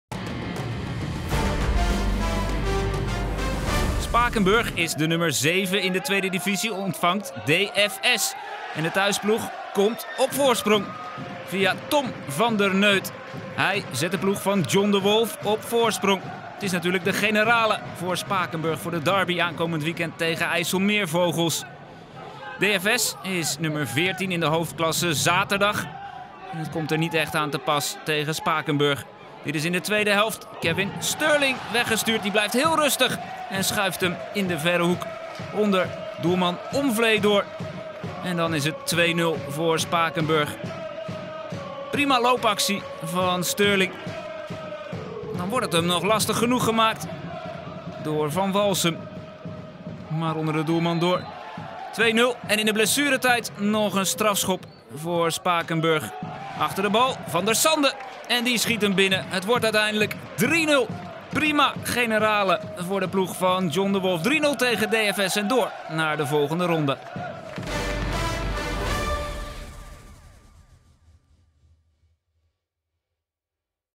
Samenvatting TOTO KNVB Beker: Spakenburg - DFS (25/9/2018)
Hoogtepunten van Spakenburg - DFS, een wedstrijd uit de eerste ronde van de TOTO KNVB Beker in het seizoen 2018/'19.